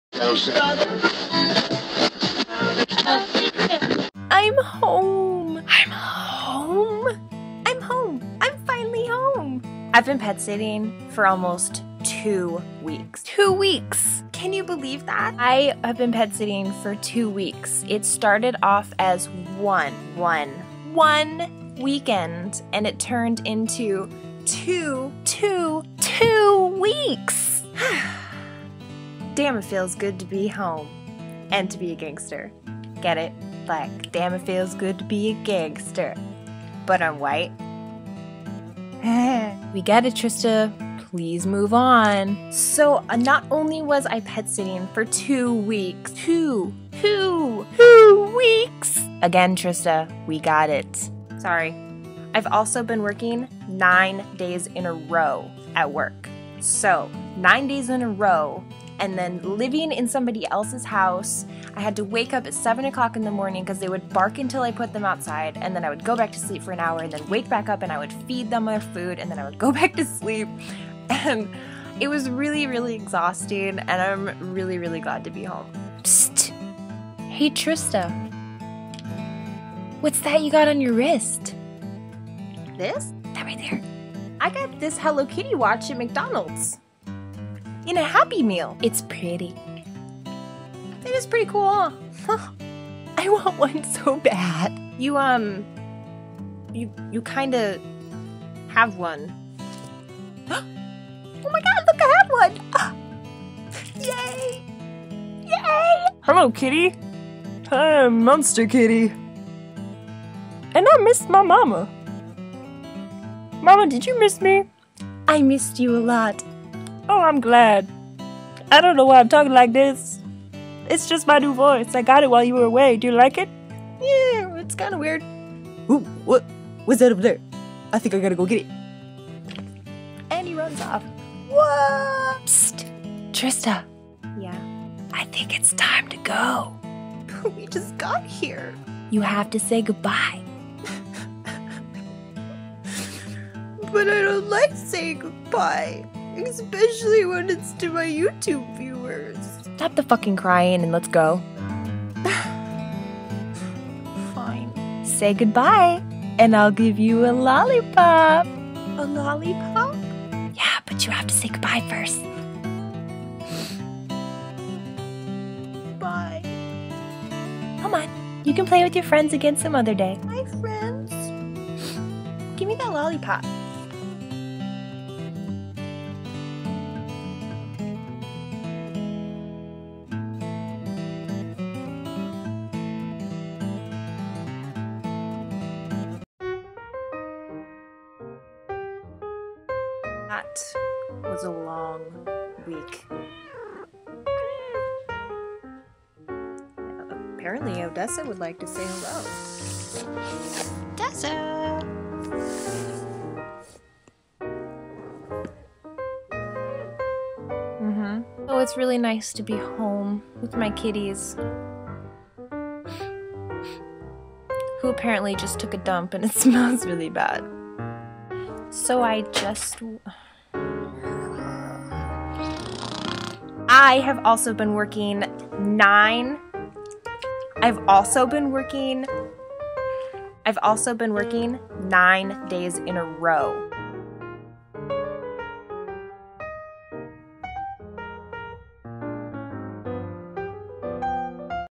Damn...it feels GOOD to be Home.
After pet sitting for nearly TWO weeks, Trista gets to go home and enjoy some time with her kitties...and youtube aka: YOU